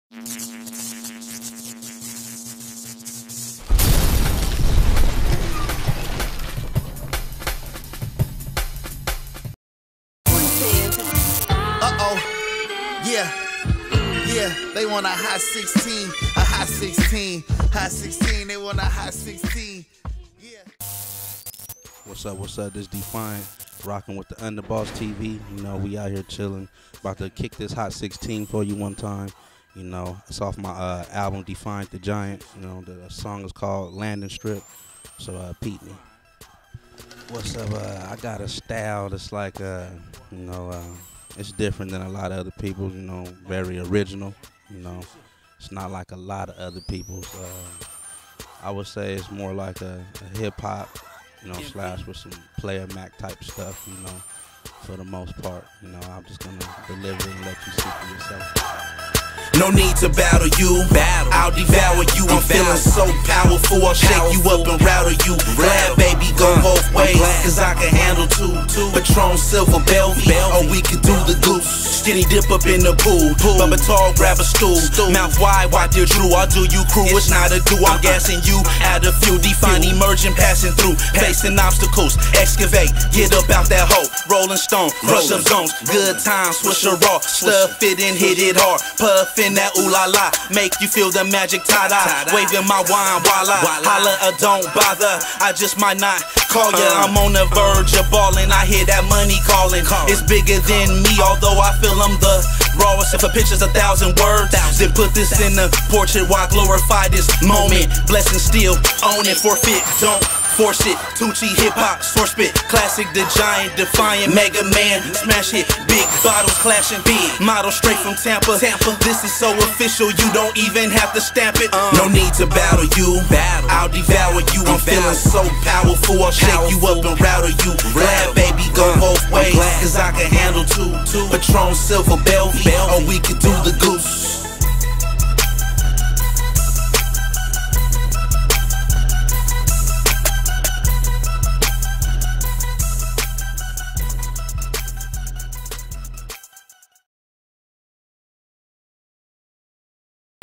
UNDABOZZ TV HOT 16- FEATURING DEFIANT DA GIANT
209 Valley Artist Defiant The giant Spits a hot 16 for Undabozz TV